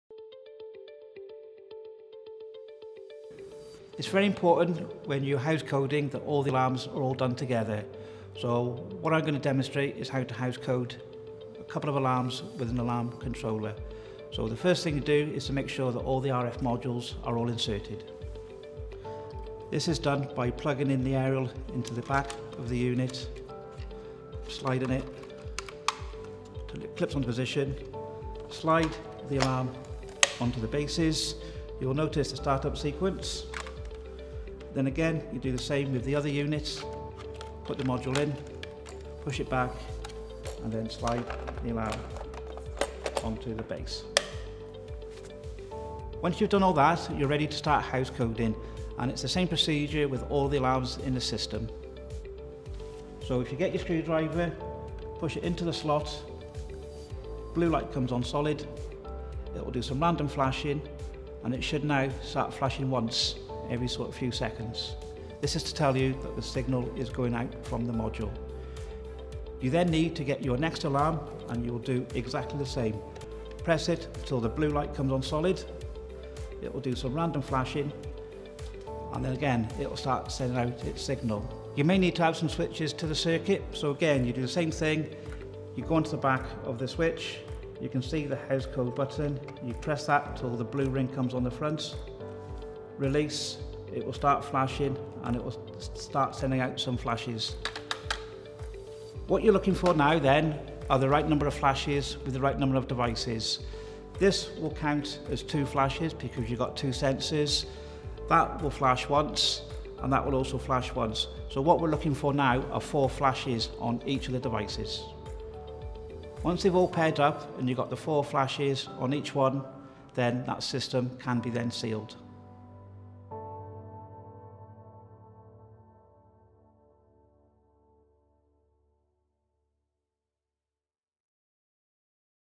RadioLINK & House Coding
Find out more on how to House Code and use Aico's RadioLINK+ wireless interconnection technology by watching this video.